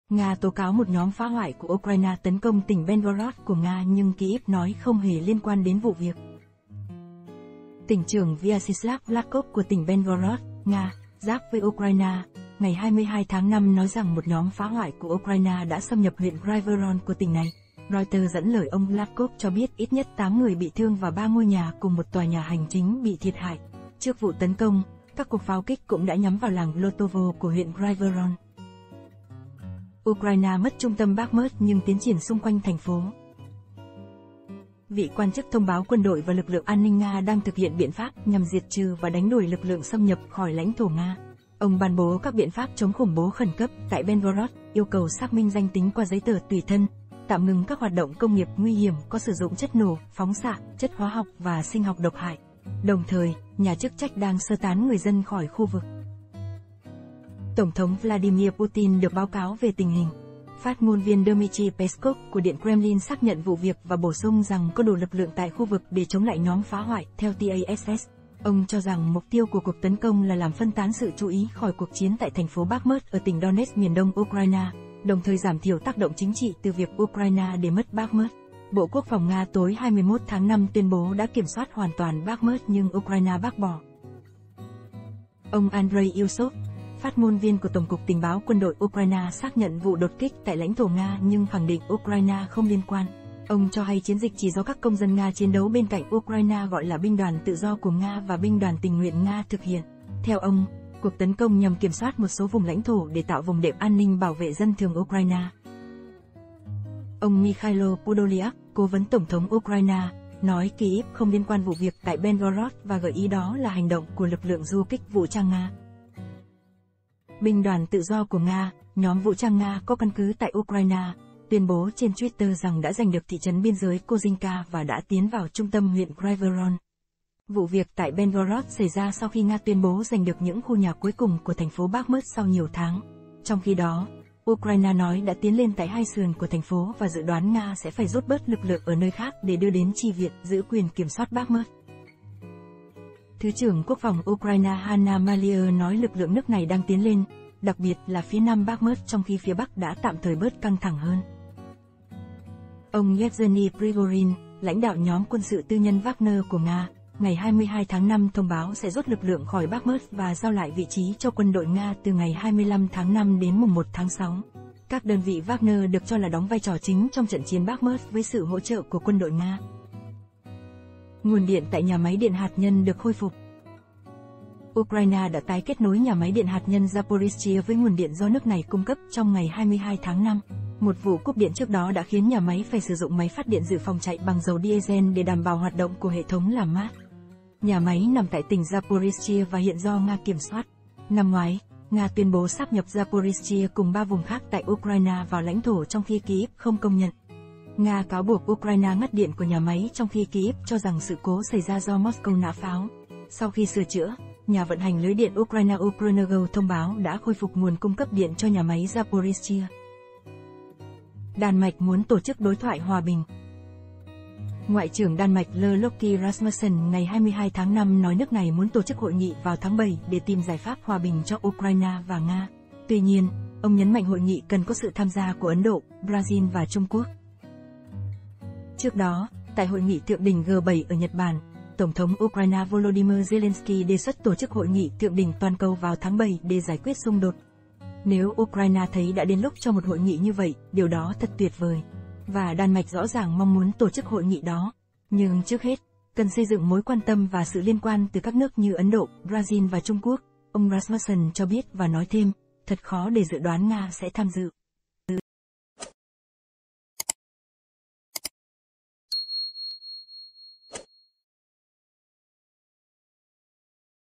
CHIẾN SỰ NGÀY 453| DIỄN BIẾN NÓNG TẠI BIÊN GIỚI NGA - UKRAINE
Tỉnh trưởng Vyacheslav Gladkov của tỉnh Belgorod (Nga) giáp với Ukraine, ngày 22  tháng 5 nói rằng một nhóm phá hoại của Ukraine đã xâm nhập huyện Grayvoron của tỉnh này. Reuters dẫn lời ông Gladkov cho biết ít nhất 8 người bị thương và 3 ngôi nhà cùng một tòa nhà hành chính bị thiệt hại. Trước vụ tấn công, các cuộc pháo kích cũng đã nhắm vào làng Glotovo của huyện Grayvoron.